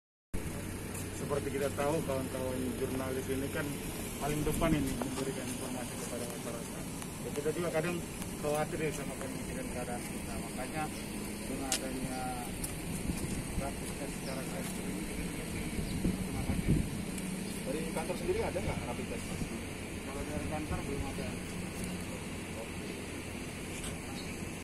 Mannttaapsss.. Warga Kepri di Gratis Rapid Test oleh Irjen.Andap
Video dari Abdul Meliala